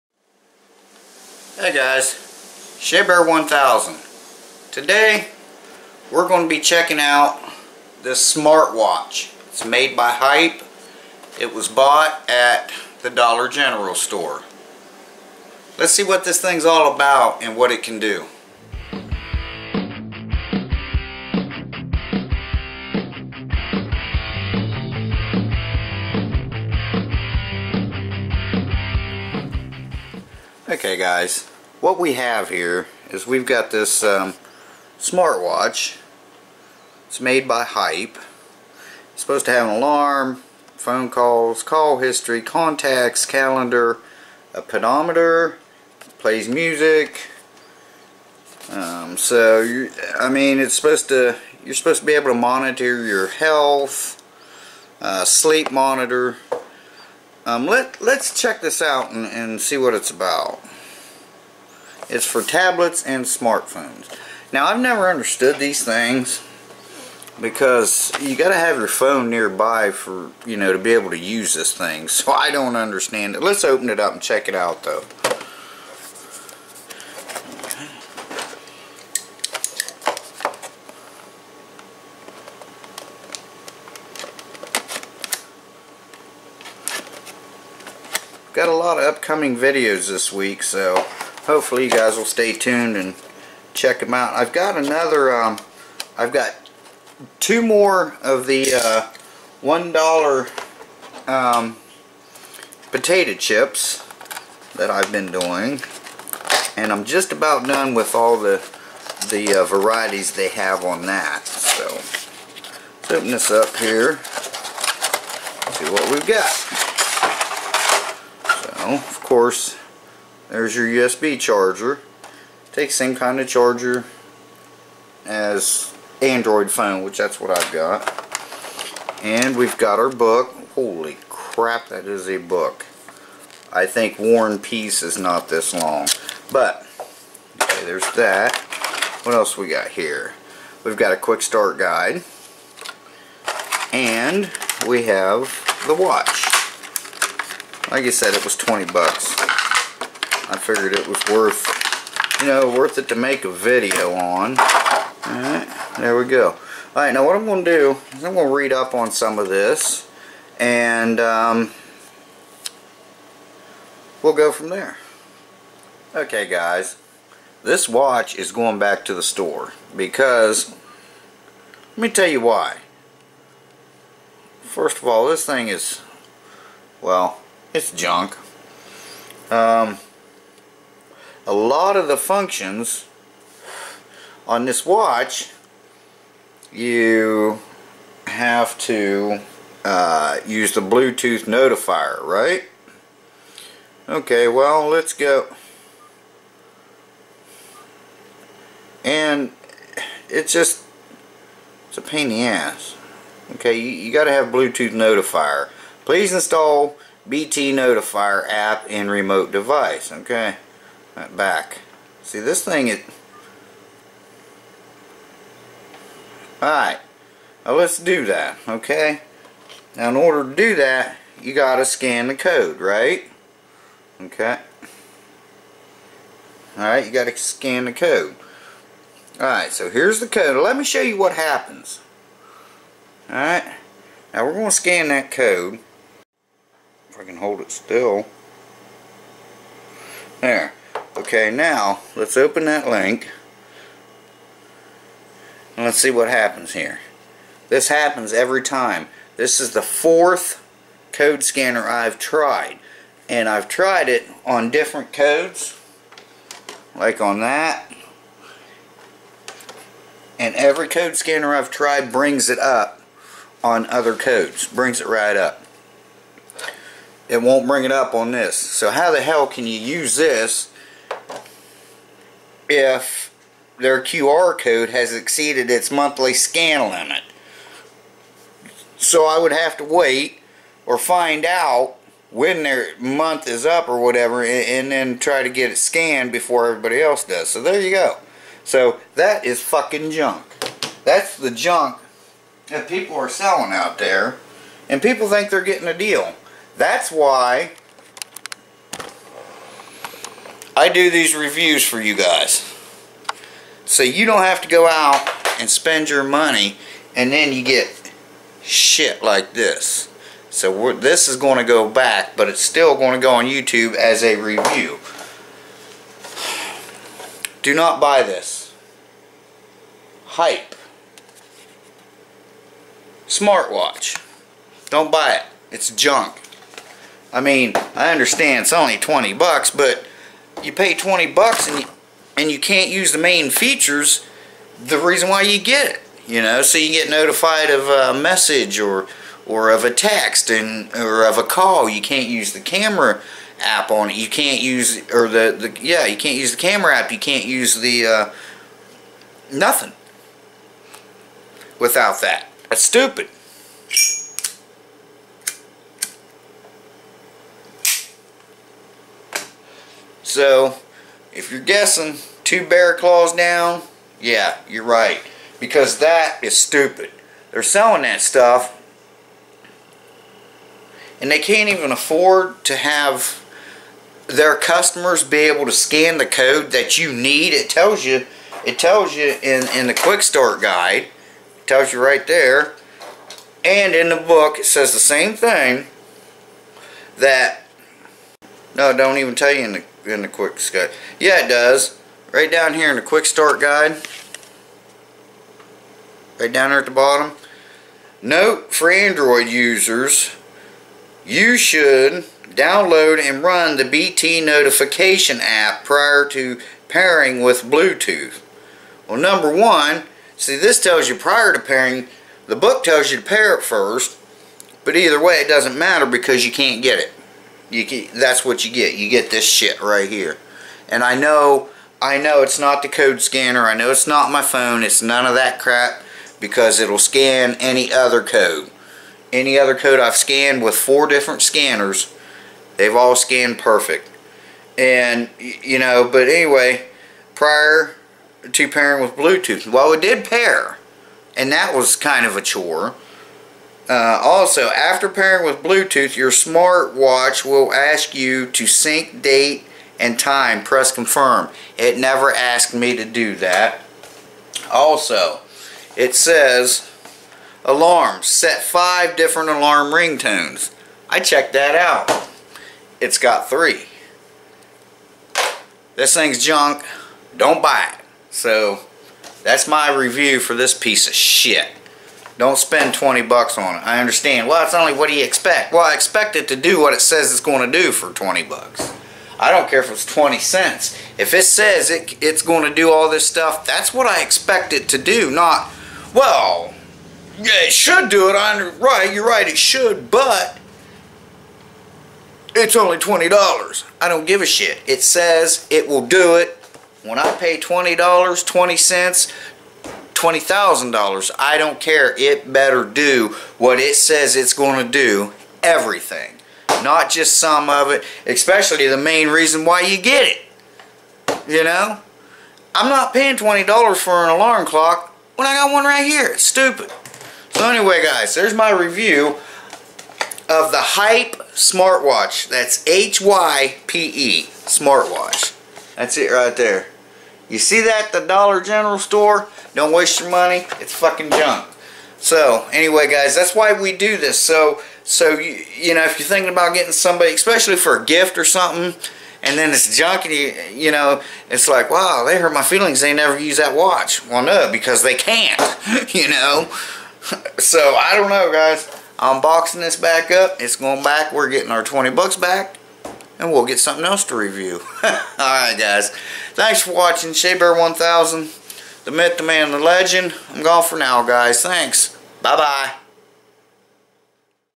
Hype Smart Watch From Dollar General Review
is this smart watch from dollar general worth 20 bucks? lets find out.